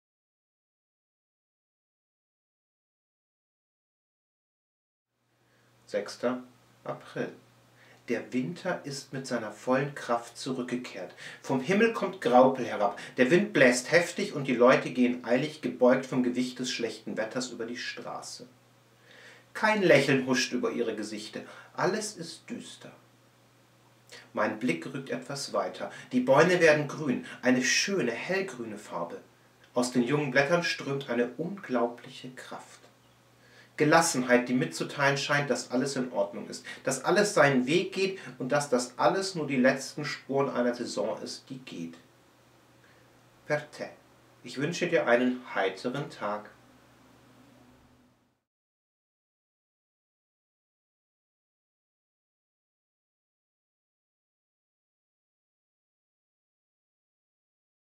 Lesung: "Per te..." - 6.April
Bei dem Buch handelt es sich um "...eine Sammlung morgendlicher Grüße, die einem helfen, jeden Tag positiv zu beginnen."

"Per te... - für dich" gibt es in verschiedenen Versionen, die alle bei Amazon erhältlich sind:

- Hardcover (DE/IT)
- Paperback und E-book (DE)
- Paperback und E-book (IT)

copyright: Verlag scheuer Autoren 2015